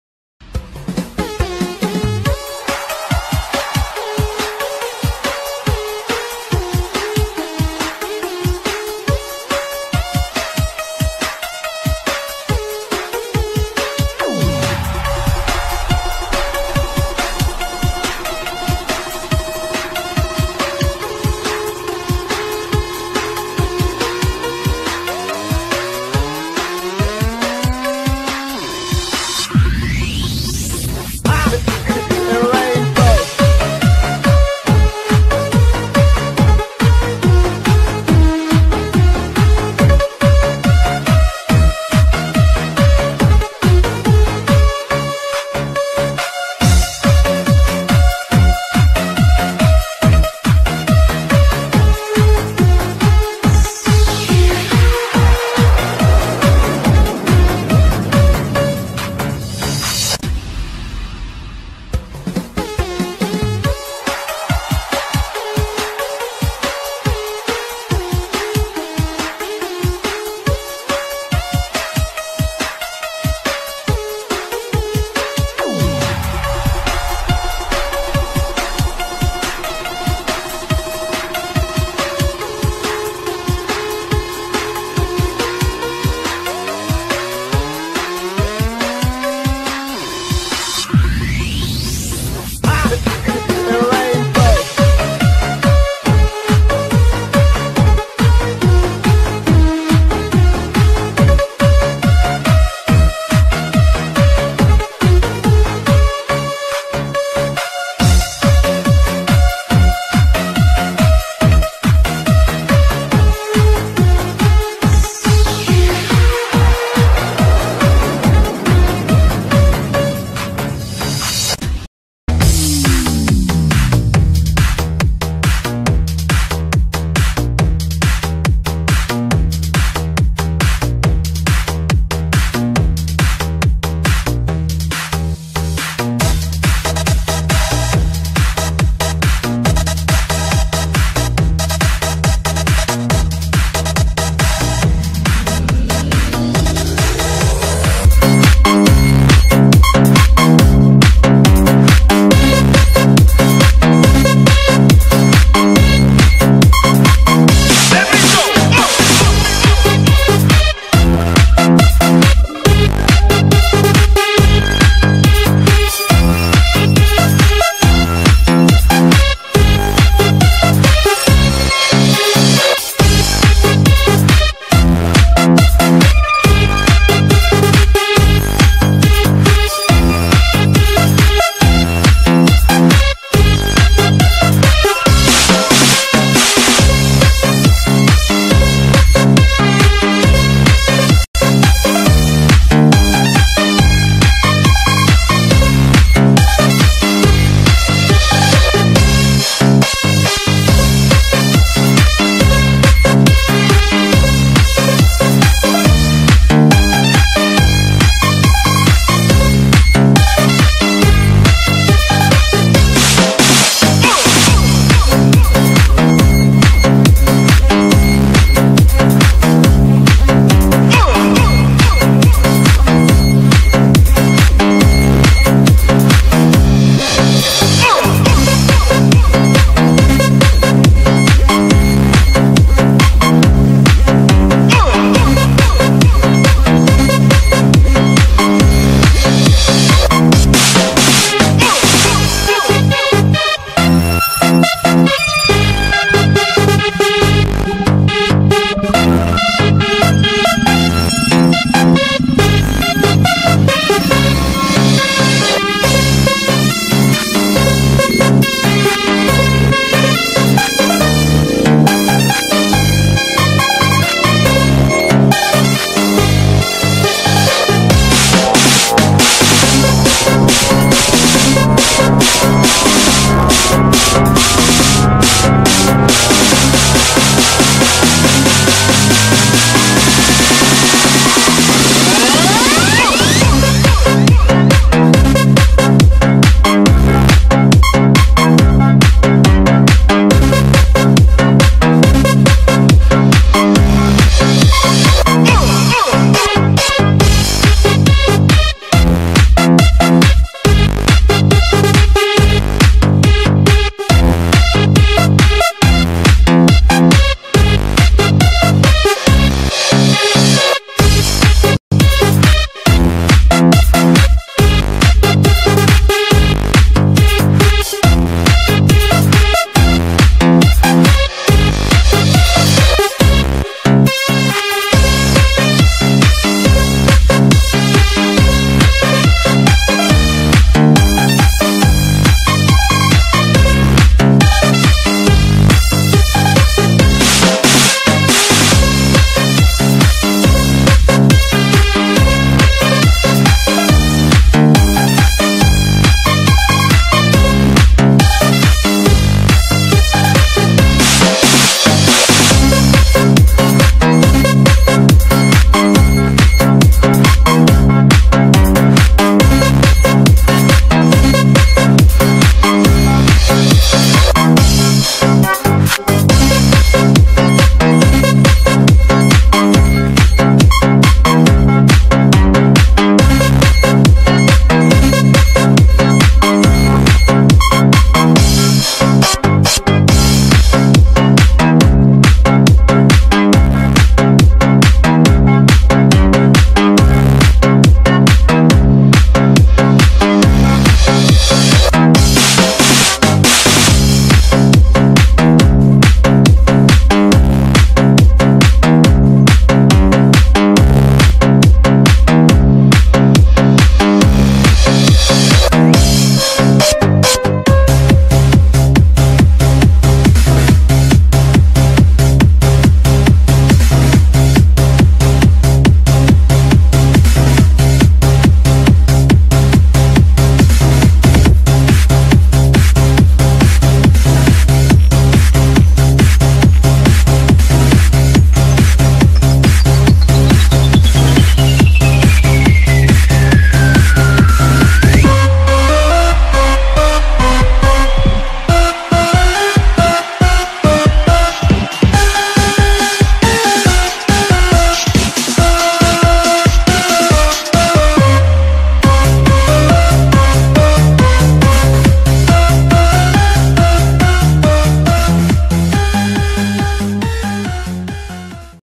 Máy Xúc KOMATSU Làm Việc, Xe Tải Cửu Long Vận Chuyển Cát | Excavator Truck | TienTube TV
Máy xúc bánh xích KOMATSU PC210 di chuyển và múc cát xây dựng lên xe ô tô tải ben Cửu Long TMT 5 Tấn trong công trường